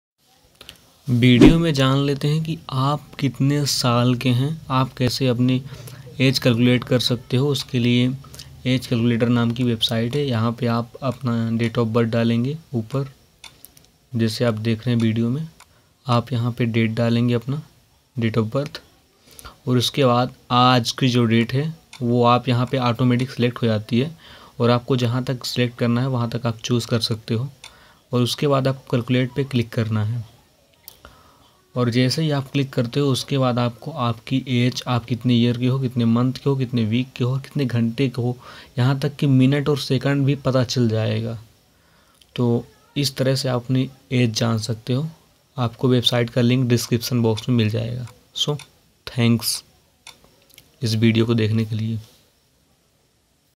Age Calculator Kaise Kare ||Date of birth kaise nikalte hai|age calculator trick
Date of birth kaise nikalte hai|Date of birth|age calculator trick|age kaise nikalte hai.
अपनी उम्र जाने आसान तरीके से /
आप कितने साल के है ।
आप कितने घंटे के है ।
यहाँ तक की आप कितने सेकंड के है वो भी चेक कर सकते है ।